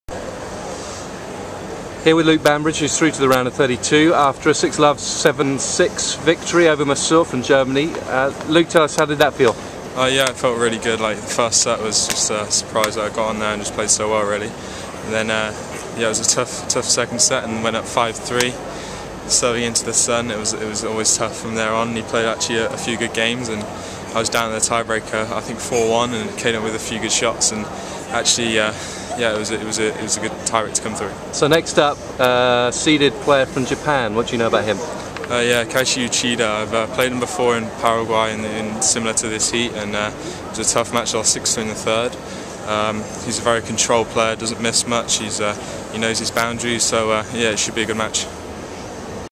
Luke Bambridge at 2012 Australian Open
British junior tennis player Luke Bambridge speaking after winning his opening boys' singles match in Melbourne.